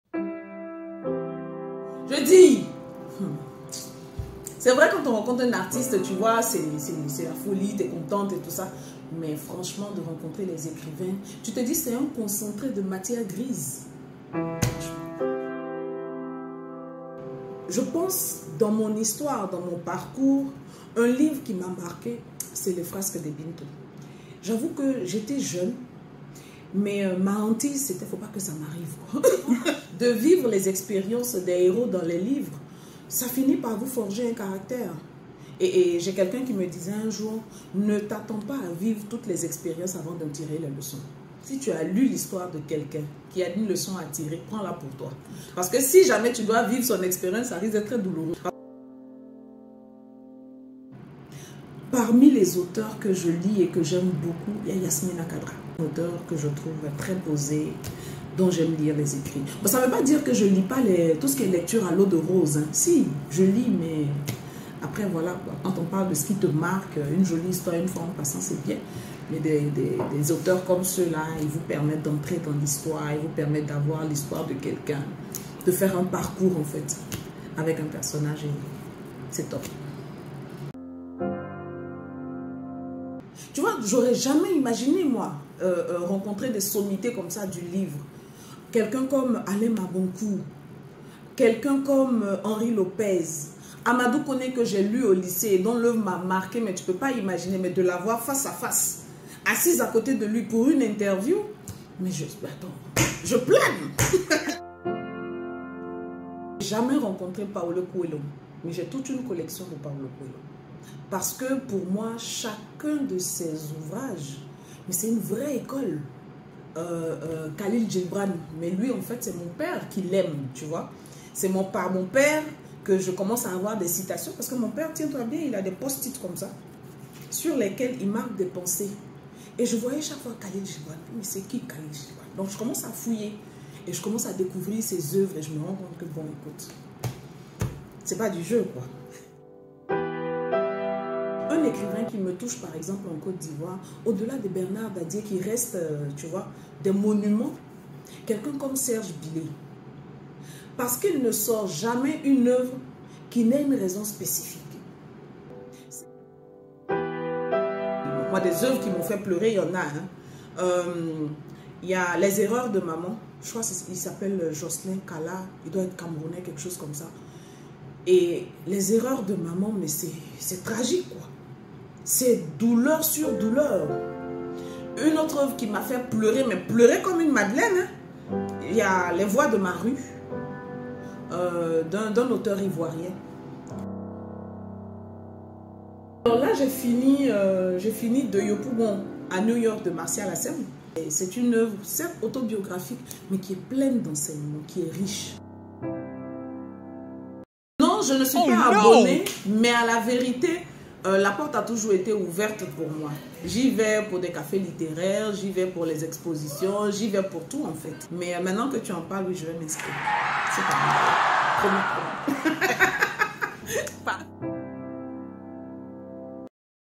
LesLivresdelaVie : Mariam Coulibaly
Pour l'IFCI, Mariam Coulibaly, directrice de Fréquence 2 revient sur les auteurs et les livres qui ont marqué sa vie.